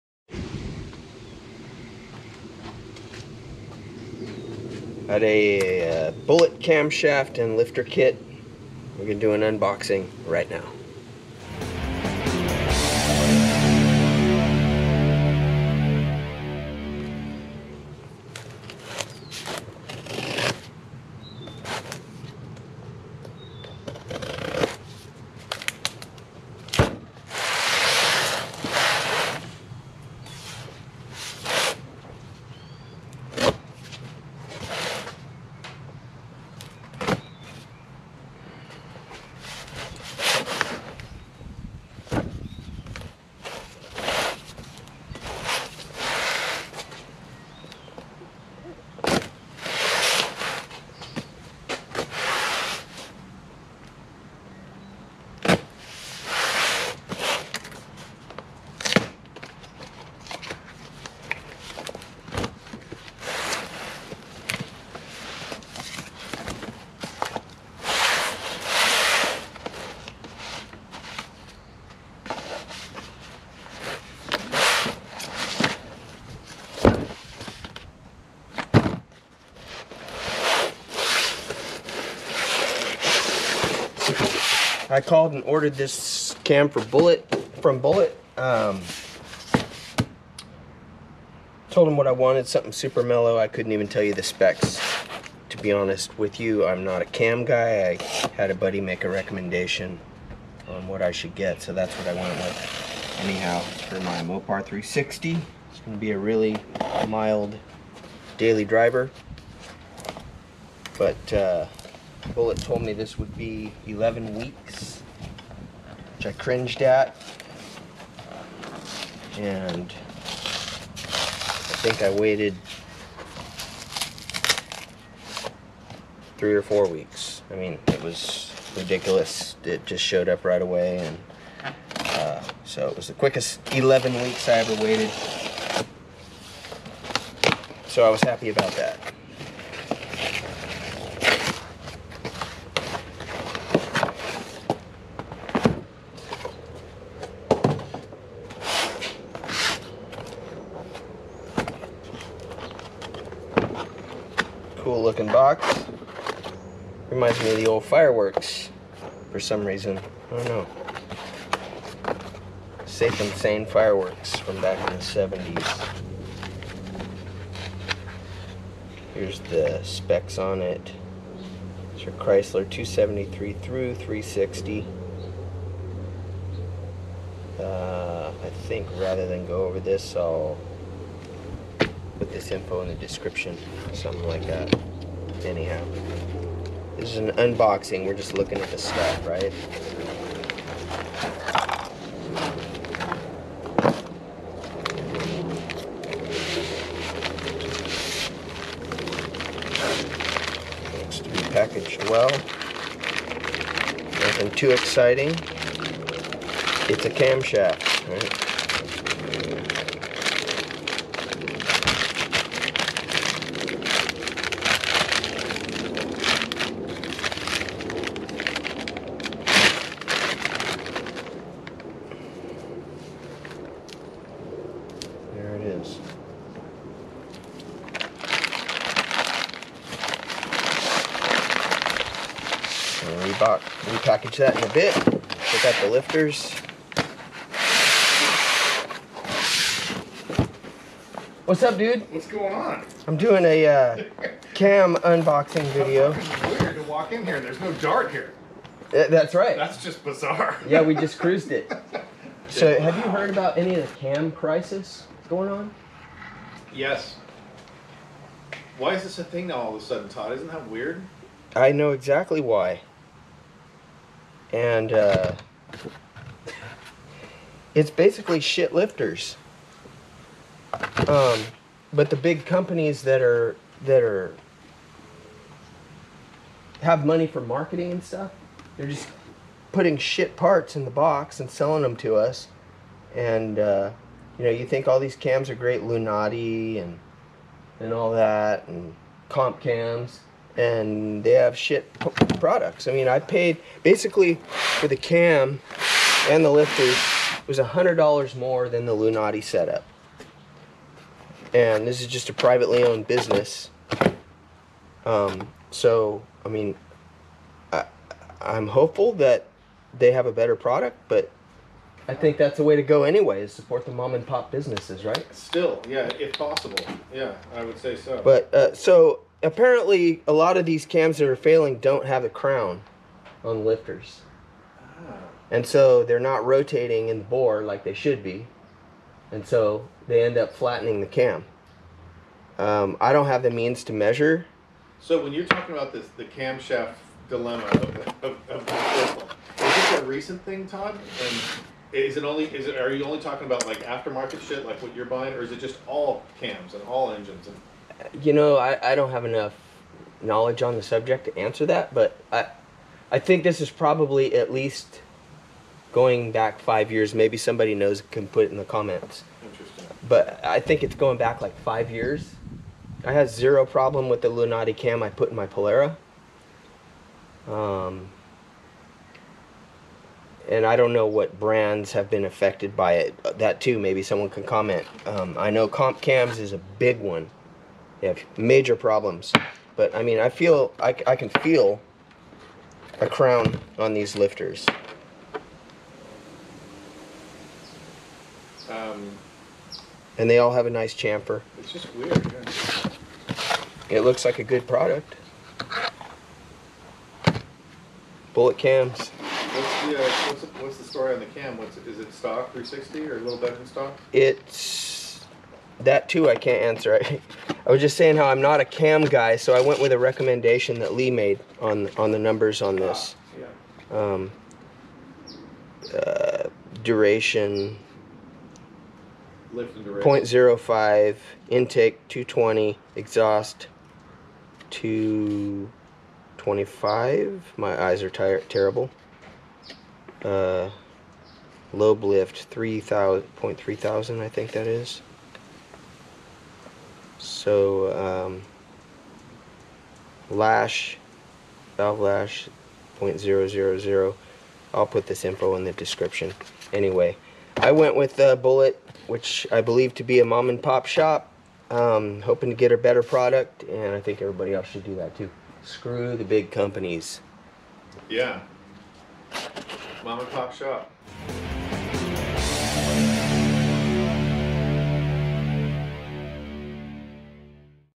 Project Mopar: Bullet Cams - Cam and Lifter Unboxing and Cam Crisis Discussion
Unboxing Bullet cam and lifter set, with some discussion with Mark on the cam crisis.

Bullet Racing Cams
Part #304000 for Mopar small blocks.